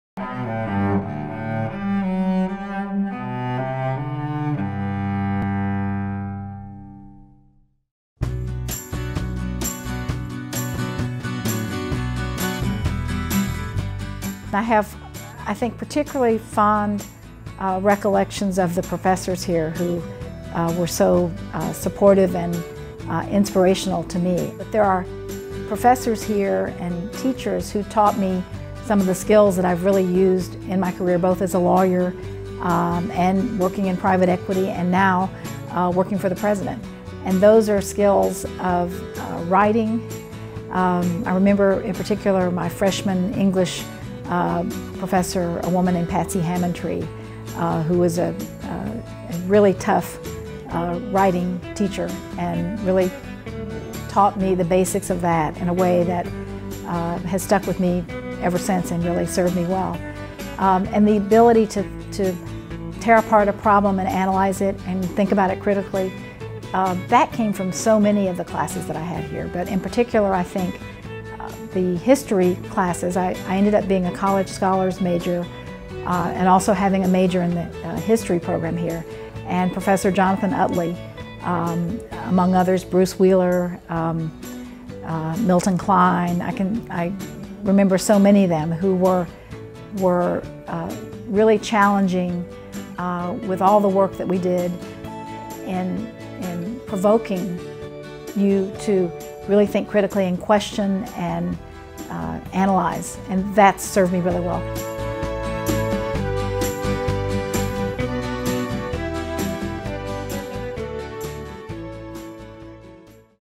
Faculty Appreciation Week: Nancy-Ann DeParle
In today's video, alumna Nancy-Anne DeParle, President Barack Obama's deputy chief of staff, reminisces about some of her favorite UT faculty.